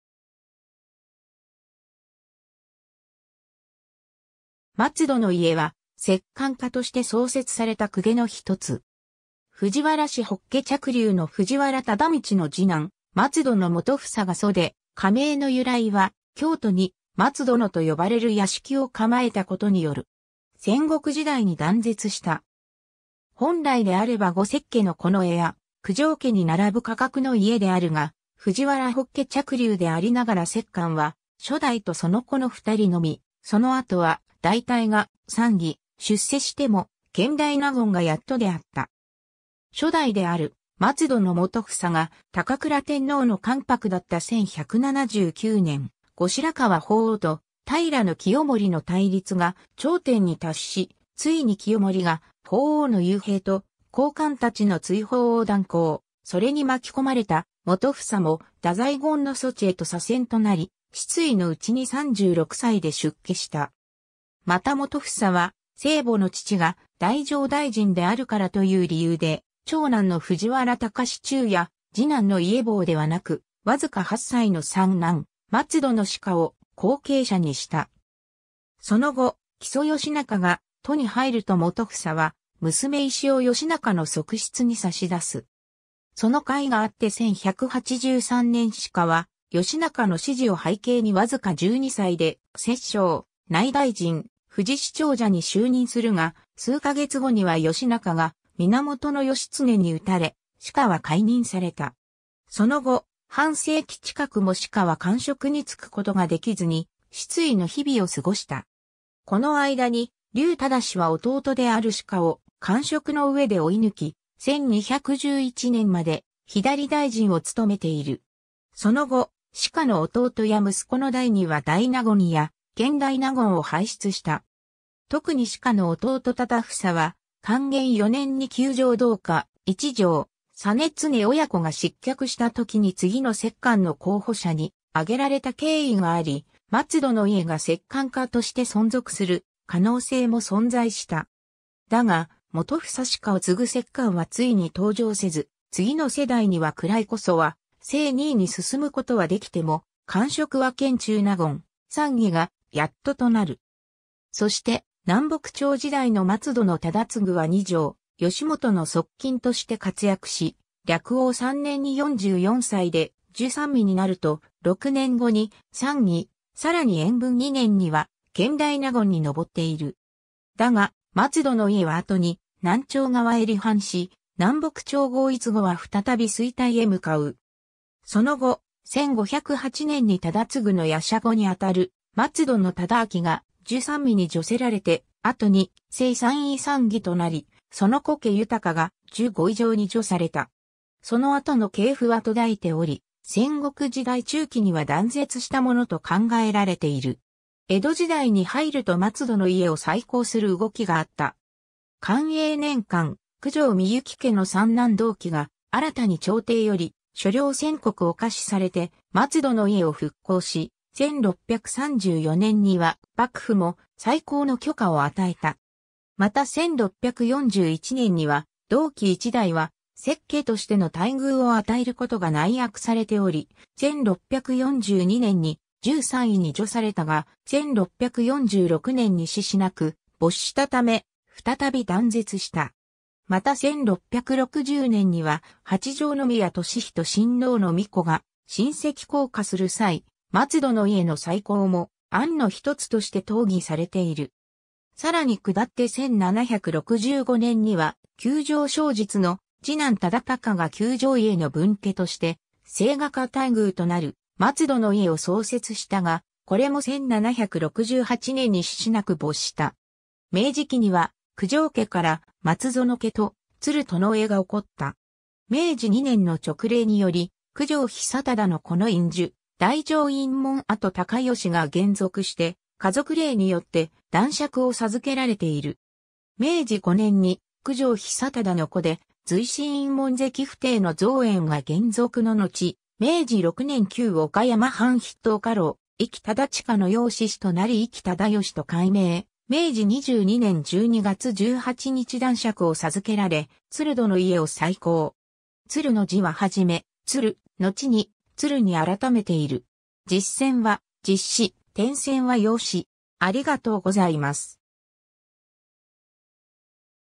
松殿家
松殿家（まつどのけ）は、摂関家として創設された公家のひとつ。
藤原氏北家嫡流の藤原忠通の次男・松殿基房が祖で、家名の由来は京都に松殿と呼ばれる屋敷を構えたことによる。
戦国時代に断絶した。
 本来であれば五摂家の近衛家・九条家に並ぶ家格の家であるが、藤原北家嫡流でありながら摂関は初代とその子の2人のみ、その後は大体が参議、出世しても権大納言がやっとであった。
 初代である松殿基房が高倉天皇の関白だった1179年、後白河法皇と平清盛の対立が頂点に達し、ついに清盛が法皇の幽閉と高官達の追放を断行、それに巻き込まれた基房も大宰権帥へと左遷となり、失意の内に36歳で出家した。
また基房は生母の父が太政大臣であるからという理由で、長男の藤原隆忠や次男の家房ではなく、わずか8歳の三男・松殿師家を後継者にした。
 その後、木曾義仲が都に入ると基房は娘伊子を義仲の側室に差し出す。
その甲斐があって1183年師家は義仲の支持を背景にわずか12歳で摂政・内大臣・藤氏長者に就任するが、数か月後には義仲が源義経に討たれ、師家は解任された。
その後、半世紀近くも師家は官職に就くことができずに失意の日々を過ごした（ちなみに師家は甥にあたる道元を養子に迎えようとして失敗している）。
この間に隆忠は弟である師家を官職の上で追い抜き、1211年まで左大臣を務めている。
 その後、師家の弟や息子の代には大納言や権大納言を輩出した。
特に師家の弟忠房は、寛元4年（1246年）に九条道家・一条実経親子が失脚した時に次の摂関の候補者に挙げられた経緯（『民経記』寛元4年9月25日条）があり、松殿家が摂関家として存続する可能性も存在した。
だが、基房-師家を継ぐ摂関はついに登場せず、次の世代には位こそは正二位に進むことはできても、官職は権中納言・参議がやっととなる。
そして南北朝時代の松殿忠嗣（基房の玄孫）は二条良基の側近として活躍し、暦応3年（1340年）に44歳で従三位になると、6年後に参議、さらに延文2年（1357年）には権大納言に昇っている。
だが、松殿家は後に南朝側へ離反し、南北朝合一後は再び衰退へ向かう。
 その後、1508年に忠嗣の玄孫にあたる松殿忠顕が従三位に叙せられて後に正三位参議となり、その子家豊が従五位上に叙された。
その後の系譜は途絶えており、戦国時代中期には断絶したものと考えられている。
 江戸時代に入ると松殿家を再興する動きがあった。
寛永年間、九条幸家の三男道基が新たに朝廷より所領1000石を下賜されて松殿家を復興し、1634年には幕府も再興の許可を与えた。
また1641年には道基一代は摂家としての待遇を与えることが内約されており、1642年に従三位に叙されたが、1646年に嗣子無く没したため、再び断絶した。
 また1660年には、八条宮智仁親王の皇子（後の広幡忠幸）が臣籍降下する際、松殿家の再興も案の一つとして討議されている。
 さらに下って1765年には、九条尚実の次男忠孝が九条家の分家として、清華家待遇となる松殿家を創設したが、これも1768年に嗣子無く没した。
 明治期には九条家から松園家と靏殿家が興った。
 明治2年（1869年）の勅令により九条尚忠の子の院主・大乗院門跡（興福寺別当大僧正）隆芳（松園尚嘉）が還俗して、華族令によって男爵を授けられている。
 明治5年（1872年）に九条尚忠の子で随心院門跡附弟の増縁が還俗ののち、明治6年（1873年）旧岡山藩筆頭家老・伊木忠恭の養嗣子となり伊木忠善と改名、明治22年（1889年）12月18日男爵を授けられ靏殿家を再興。
「つる」の字ははじめ「靏」、のちに「鶴」に改めている。
 実線は実子、点線（縦）は養子。